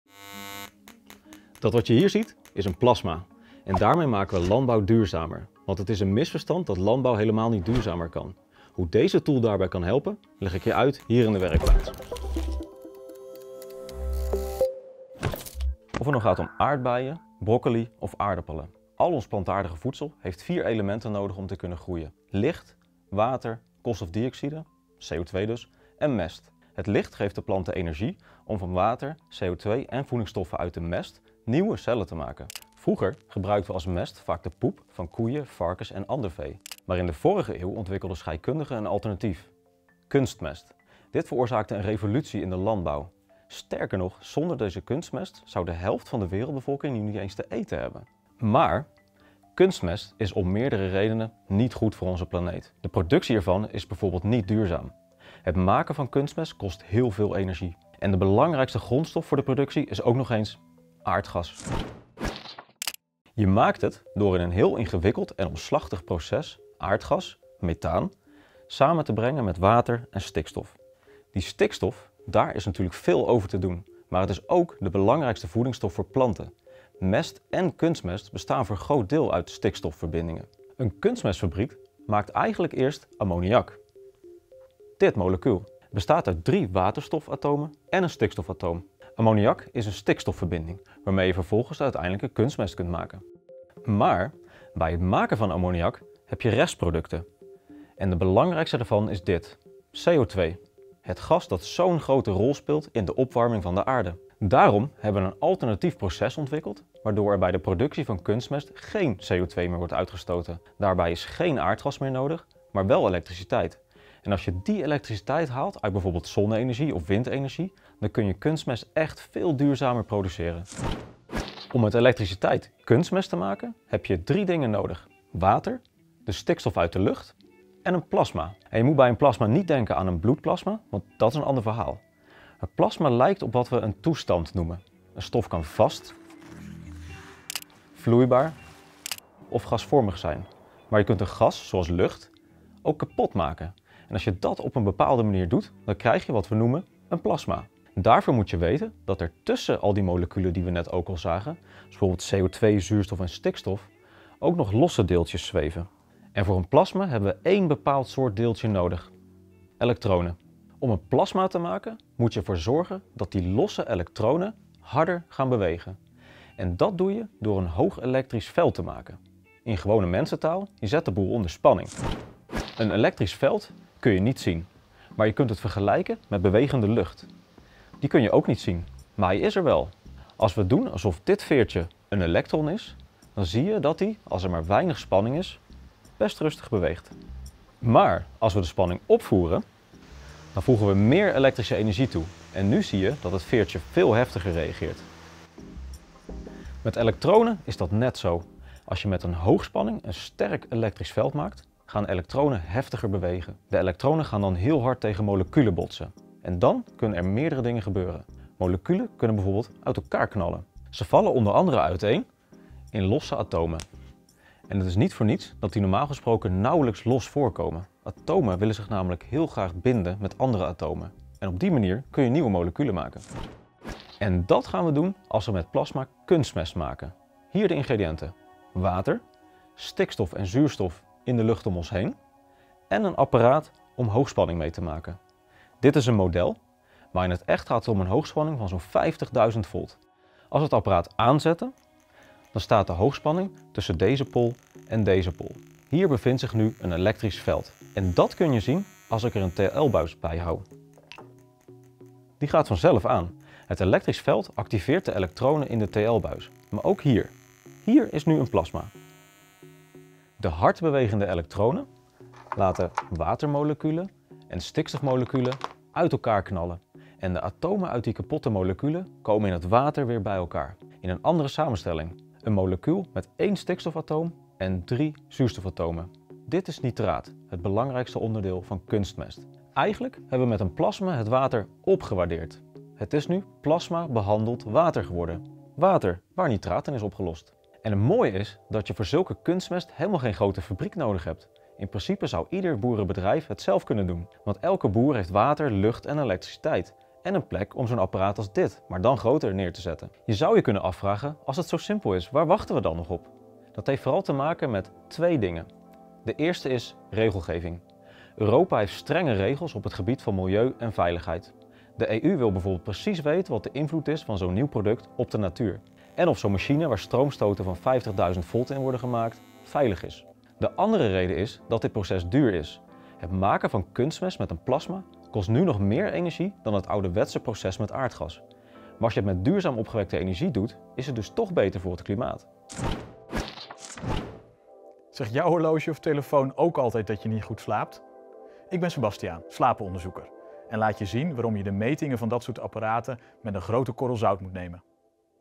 Hoe maakt 50.000 volt de landbouw duurzamer?
Zonder kunstmest heeft de helft (!) van de wereldbevolking geen eten. Maar het is geen duurzaam middel: kunstmestfabrieken stoten ontzettend veel CO2 uit (koolstofdioxide) - het broeikasgas dat hoofdverantwoordelijk is voor de opwarming van de aarde. Daarnaast is kunstmest slecht voor de bodem.
Dat bodemprobleem kan de intensieve landbouw nog niet oplossen, maar de productie van kunstmest kan wél duurzamer: met hoogspanning.
Met stroomstoten van 50.000 volt kun je een 'plasma' maken waarin nitraat ontstaat: het belangrijkste onderdeel van kunstmest. Hoe dat precies in z'n werk gaat, legt Tom Huiskamp (TUe) je uit in deze aflevering van de Werkplaats.

0:00 Waarom hebben we mest of kunstmest nodig?
0:49 Waarom is kunstmest niet duurzaam?
2:01 Hoe maak je met elektriciteit kunstmest?
4:41 Waarom maken we nog geen kunstmest met plasma?